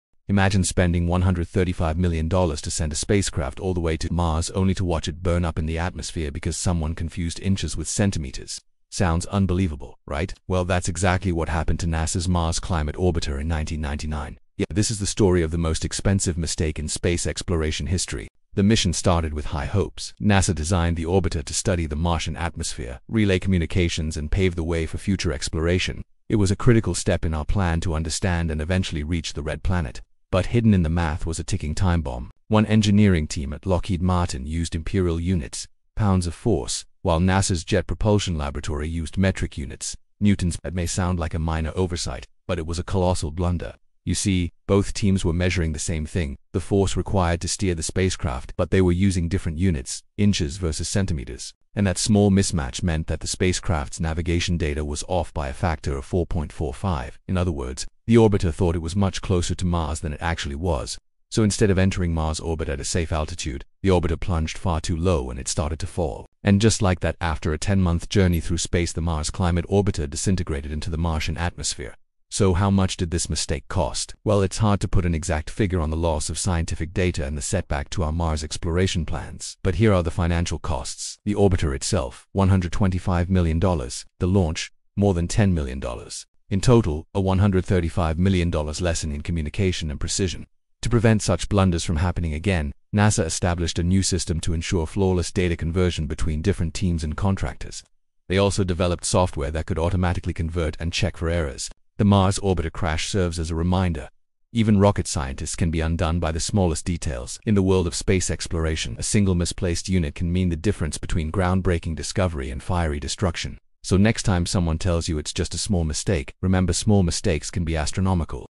The $135 Million NASA Blunder Lost in Space… Because of Inches vs Centimeters!
In 1999, NASA lost the Mars Climate Orbiter in one of the most jaw-dropping mistakes in space history. After a flawless launch, the spacecraft was supposed to orbit Mars and send back vital climate data. Instead, it vanished in the Martian atmosphere… all because one team used imperial units and the other used metric units.

This tiny oversight—pounds vs. newtons—led to a $135 million disaster. A story that proves even rocket scientists aren’t immune to simple mistakes.

👉 Discover how this happened, why it matters, and what NASA learned from one of the most expensive unit mix-ups in history.